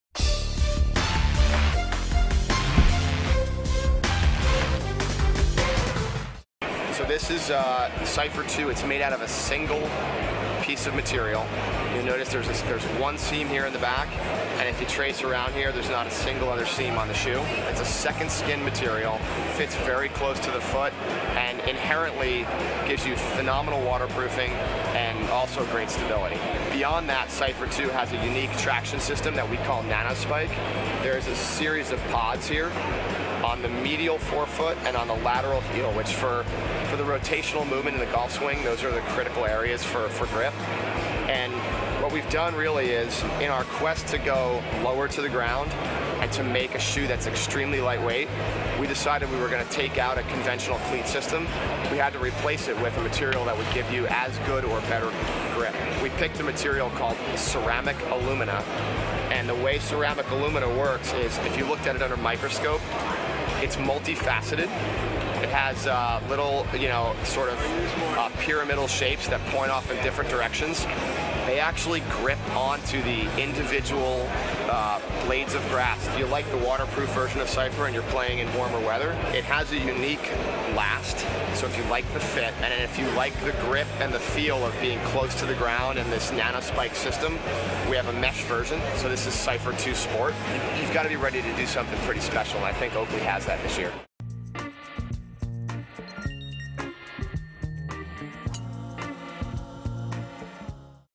Oakley Golf - Cipher 2
Oakley introduces its latest golf shoe called the Cipher 2.  Golf Life media had the chance to talk to Oakley at the 2013 PGA Merchandise show in Orlando to learn more about these new Oakley Shoes.